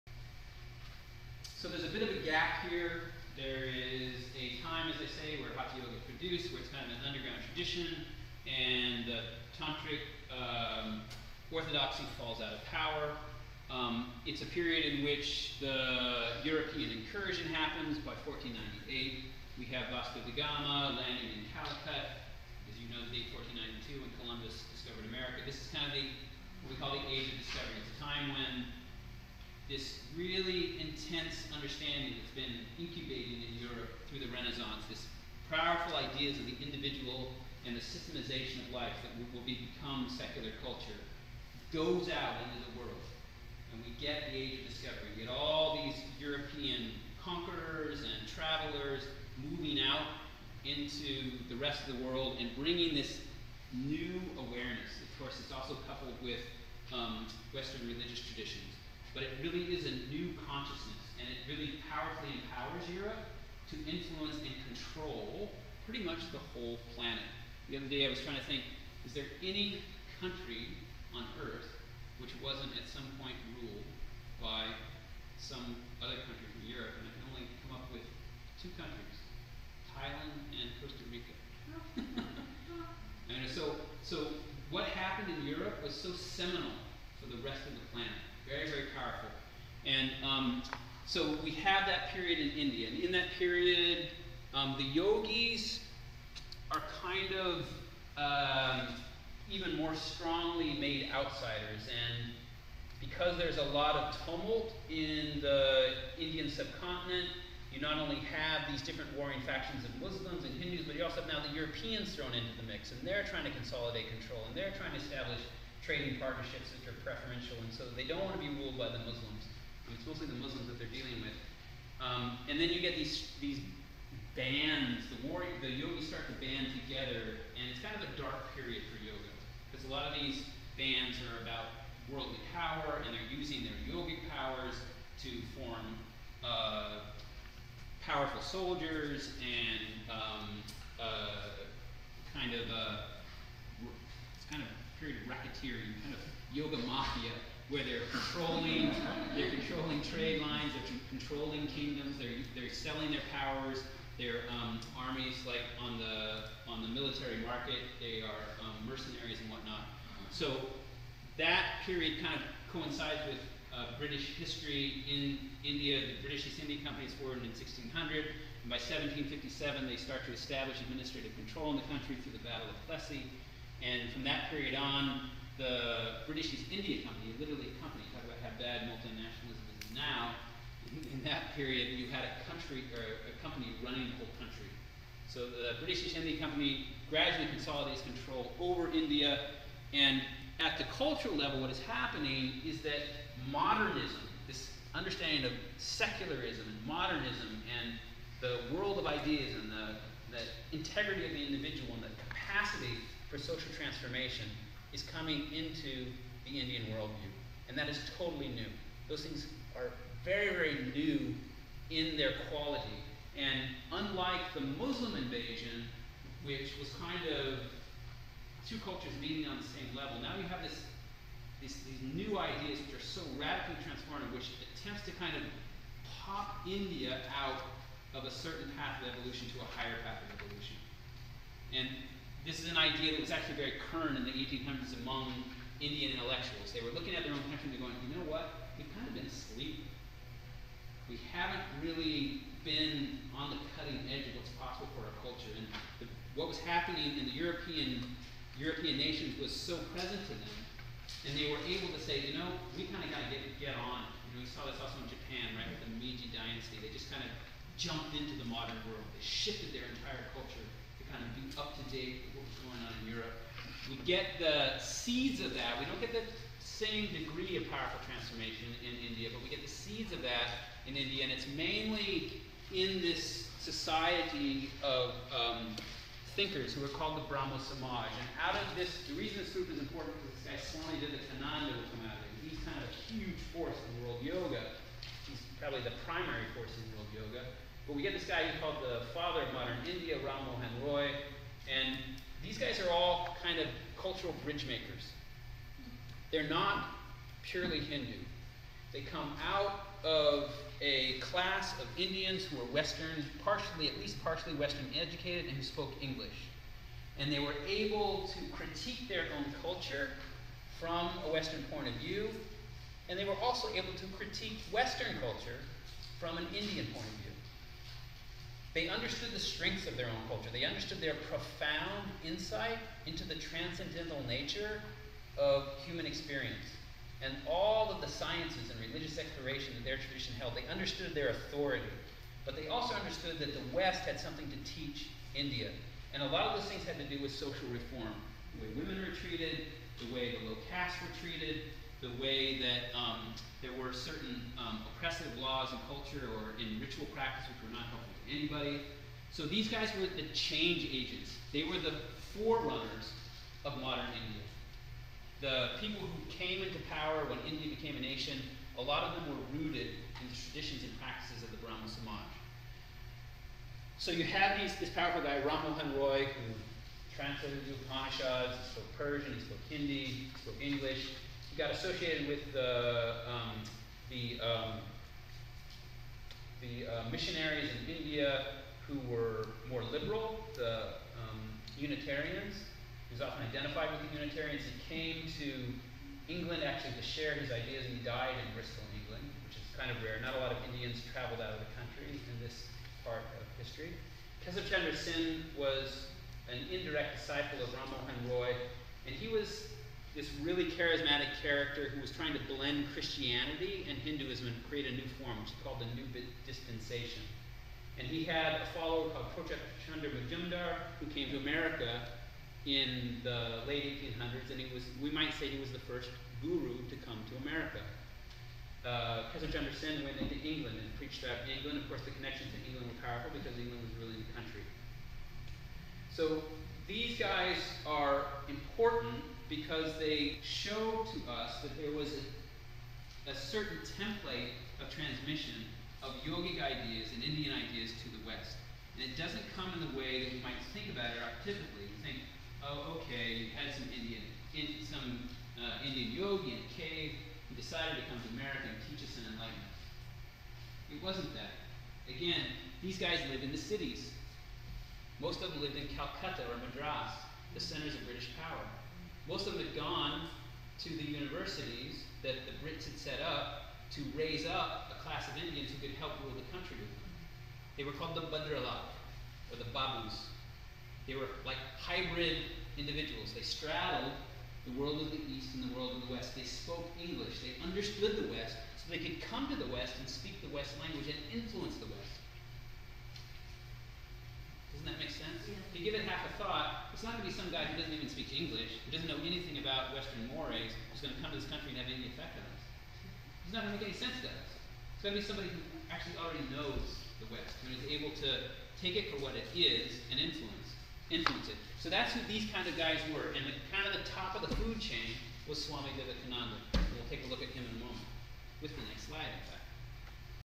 The First Transnational Indian Gurus
In this video, we look at the shifts in 1800s India that created the first important teachers who came to the West.